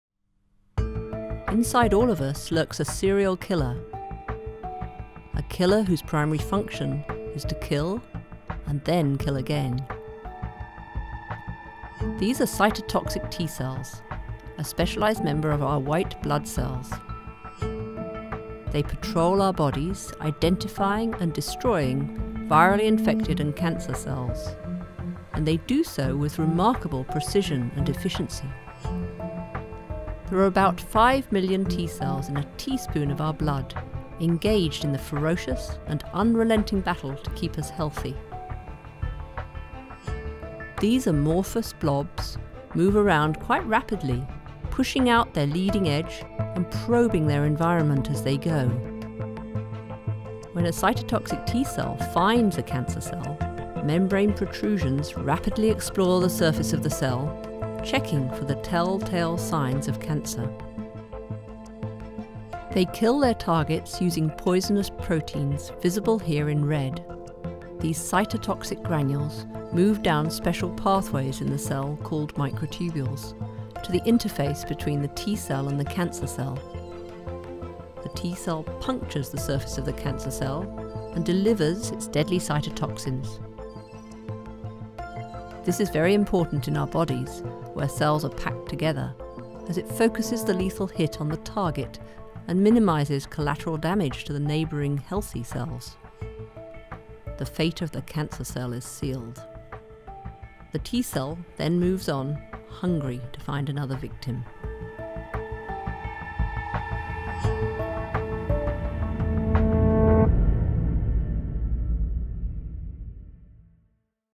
Killer T Cell: The Cancer Assassin
How does a Killer T Cell Kill its target?

Our new film captures the behaviour of cytotoxic T cells – the body’s ‘serial killers’ – as they hunt down and eliminate cancer cells before moving on to their next target.